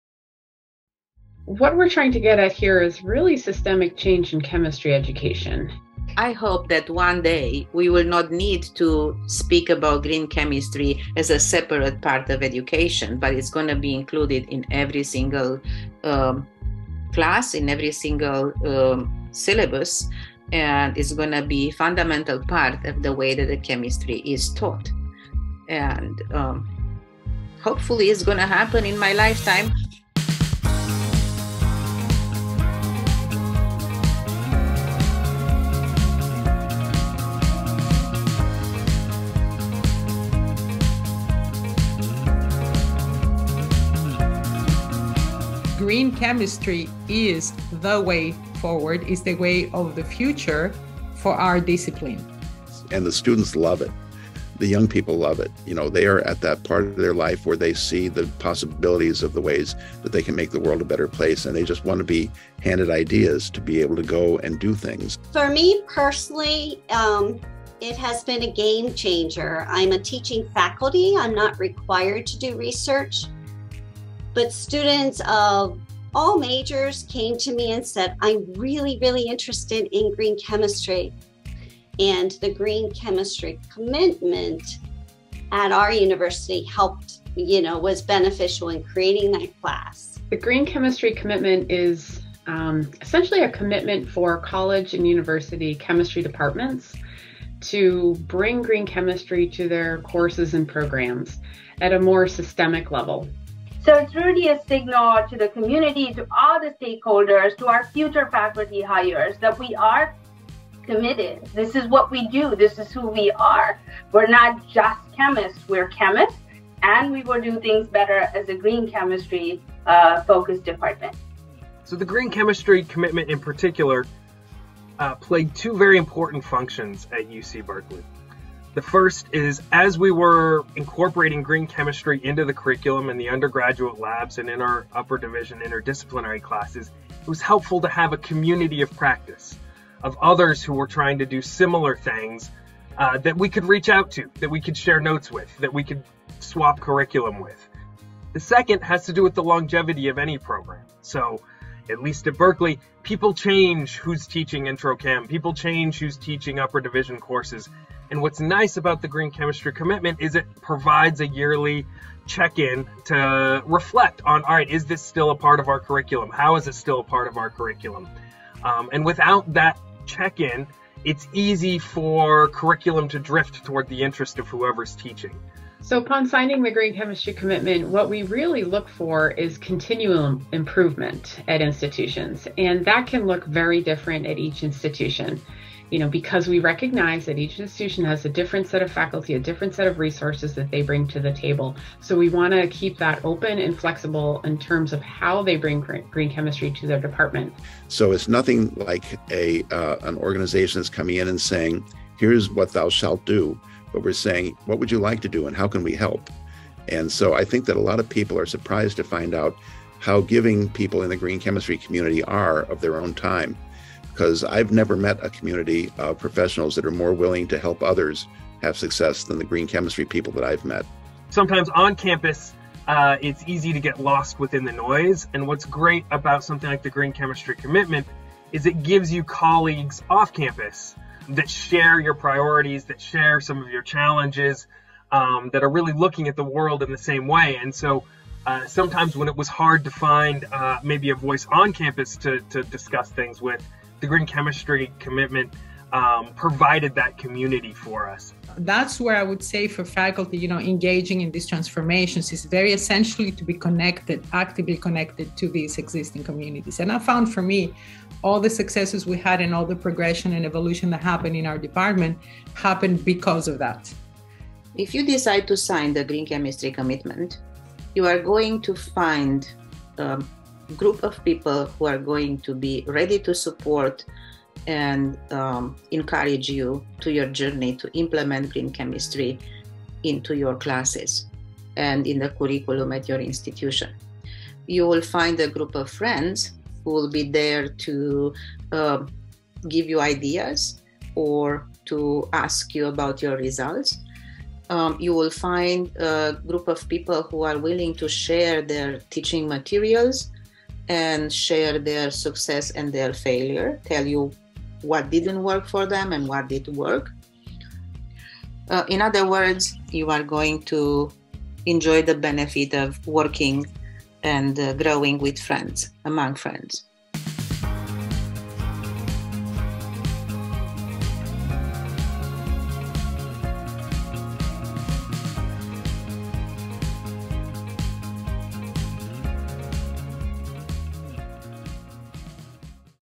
Celebrating 10 years of the Green Chemistry Commitment
Celebrate with Beyond Benign and our participating institutions 10 years of the Green Chemistry Commitment (GCC)! The GCC supports and empowers individuals and institutions to provide a systemic change in chemistry education through green chemistry!